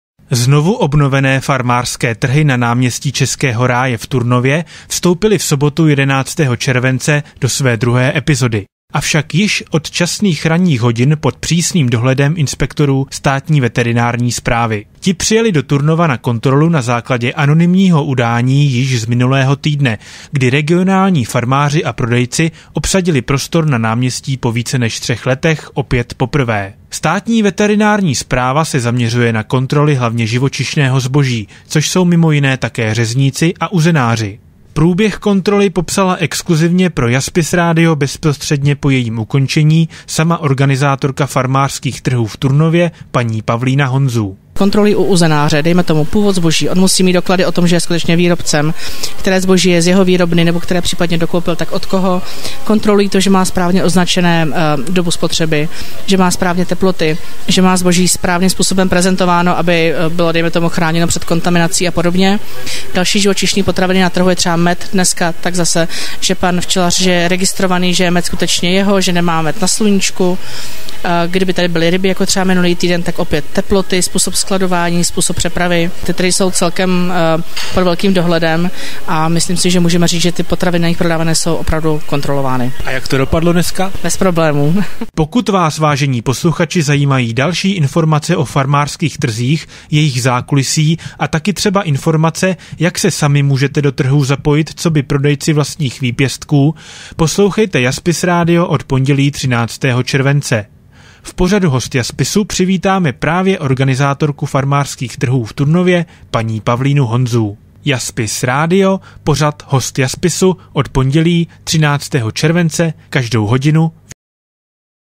Kauza JASPISU - Farmářské trhy pod kontrolou
Znovuobnovené Farmářské trhy v Turnově kontrolovali inspektoři Státní veterinární správy. Podle dostupných informací na základě anonymního udání adresovaného z Turnova již minulý týden.

Státní veterinární správa se zaměřuje hlavně na produkty živočišné výroby, kterou nabízejí také mimo jiné i řezníci a uzenáři.

Kauzu JASPISU připravil Jan Fišer